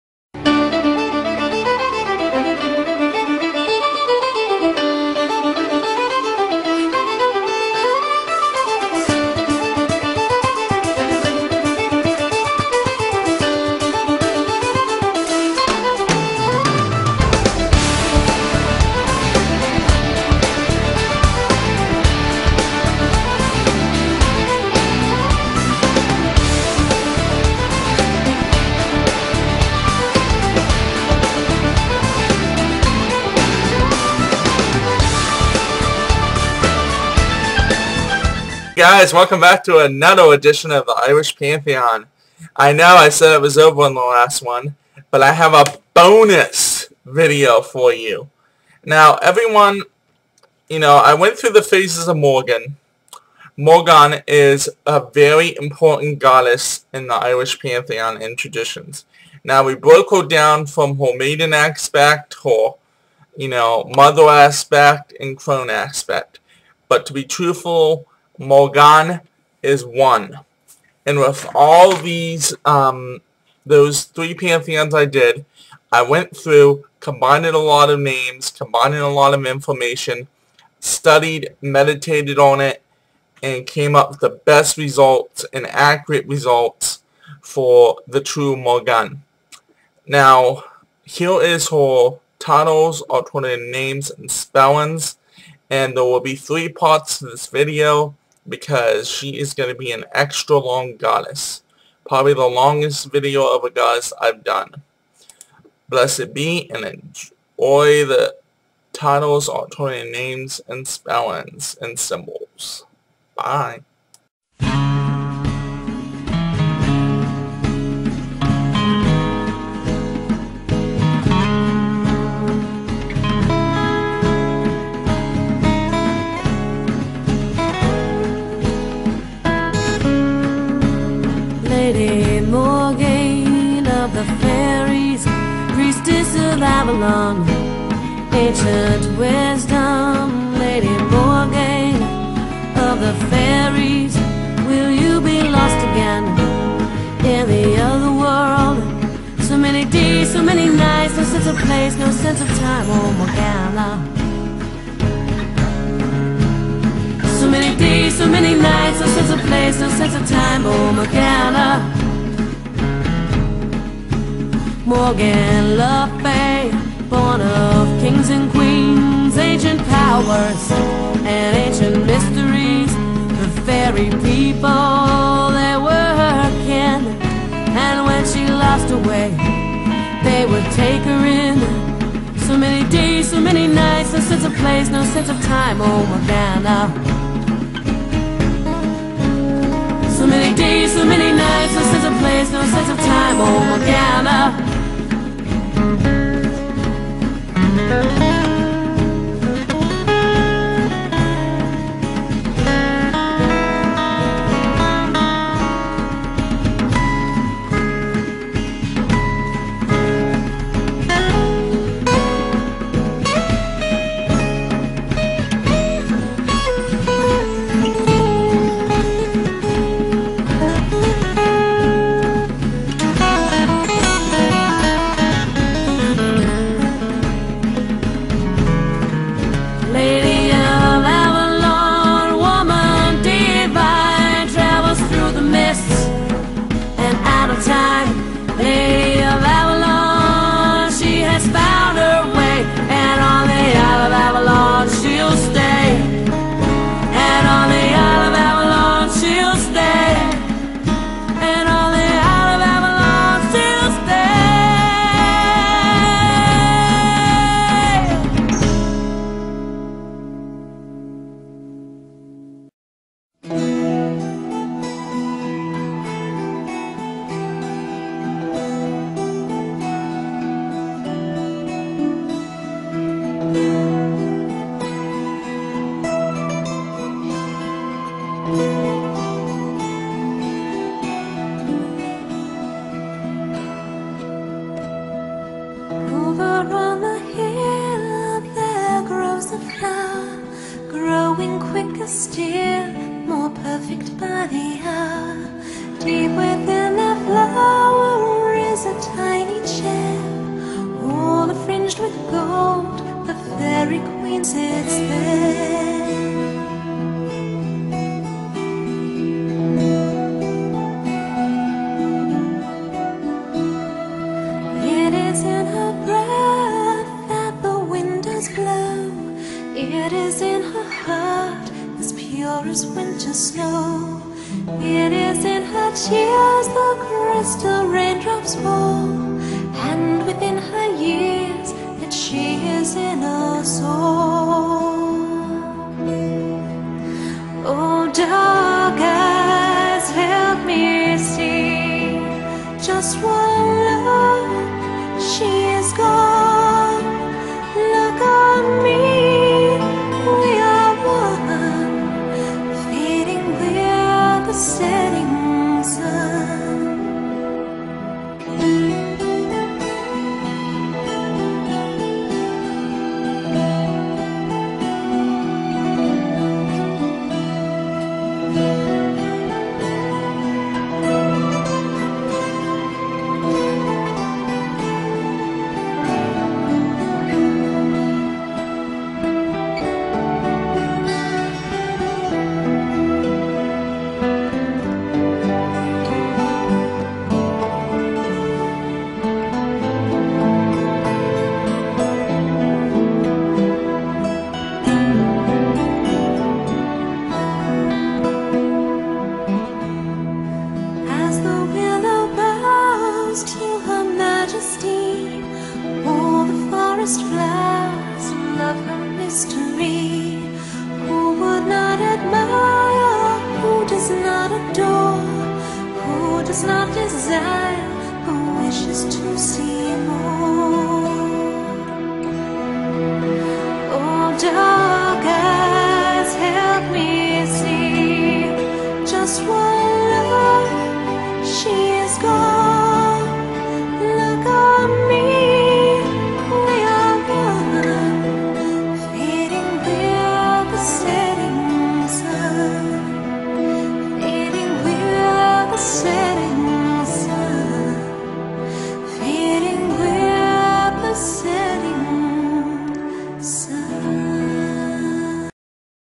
Goddess Morgan Part I of 2
Morgan is the 11th and last Goddess I will be talking and showing pictures as I talk.